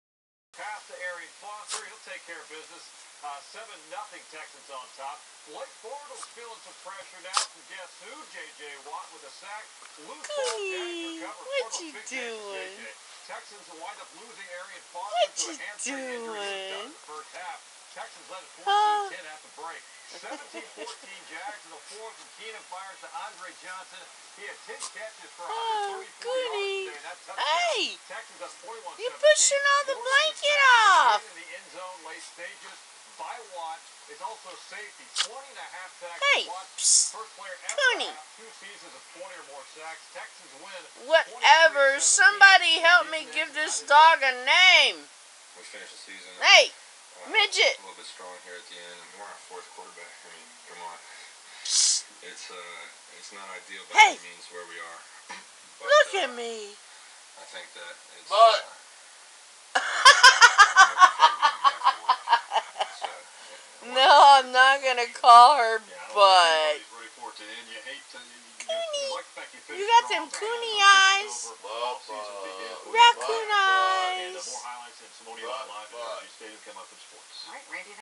New Puppy! Still no name but she is the sweetest thing ever!
Our Pups are still unsure about the new addition and its still a stand off for now...